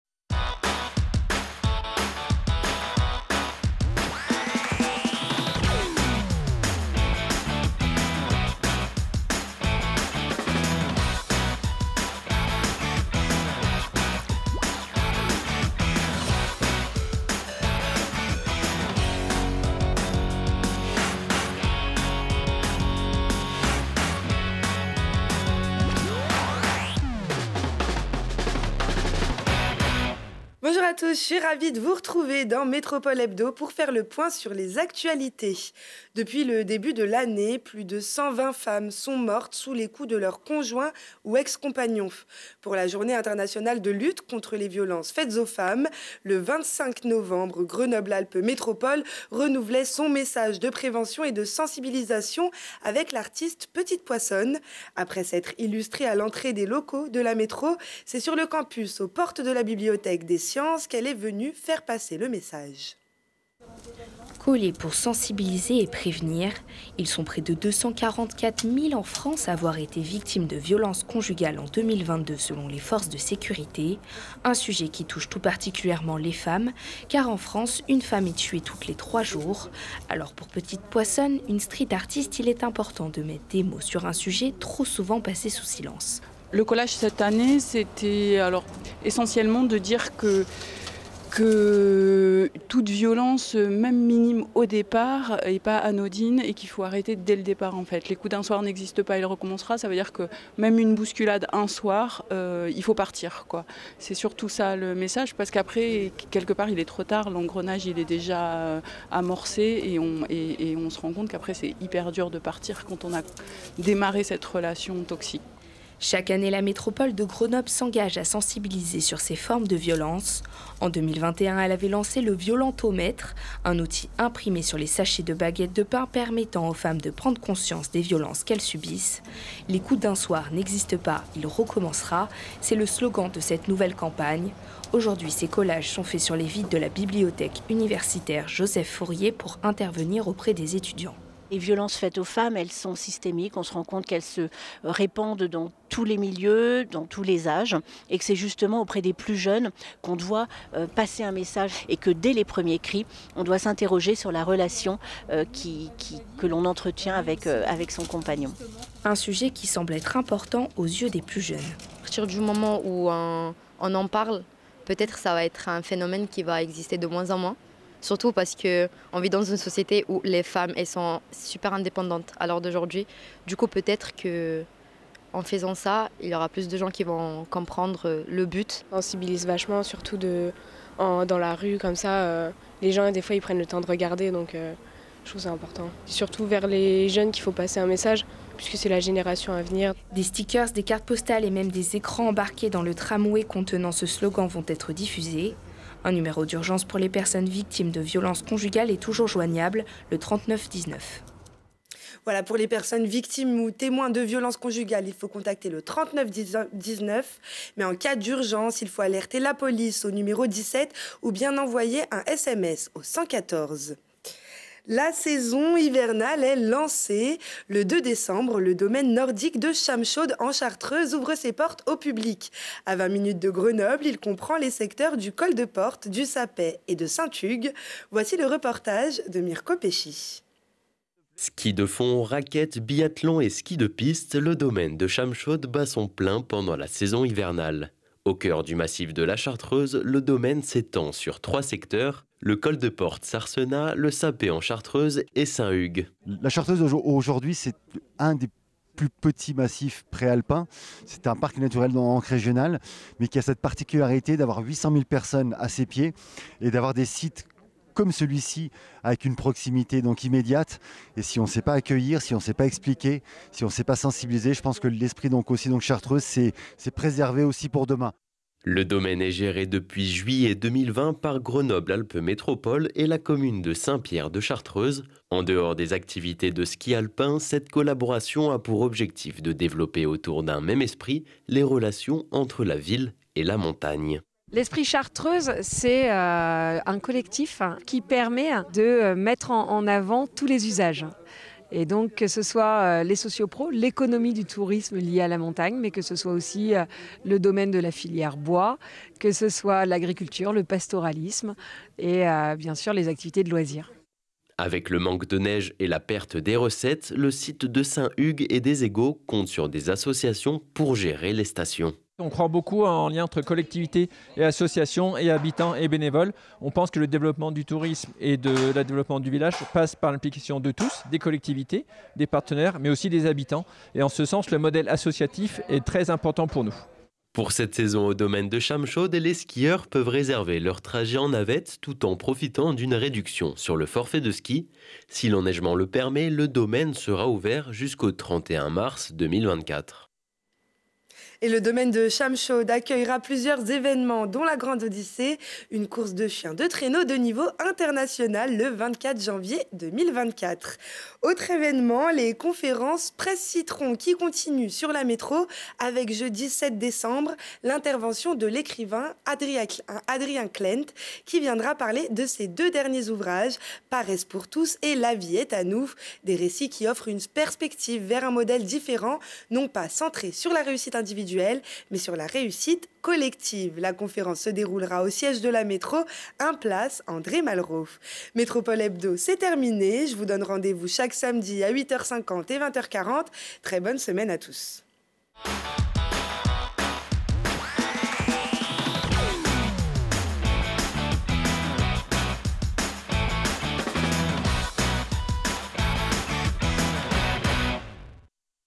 METROPOLE HEBDO - 02/12/23 - Femmes et Chamechaude
Pour la journée internationale de lutte contre les violences faites aux femmes le 25 novembre, Grenoble Alpes Métropole renouvelait son message de prévention et de sensibilisation avec l'artiste Petite Poissone.
La saison hivernale est lancée, le domaine nordique de Chamechaude en Chartreuse ouvre ses portes au public. À 20 minutes de Grenoble, il comprend les secteurs du Col de Porte, du Sappey et de Saint Hugues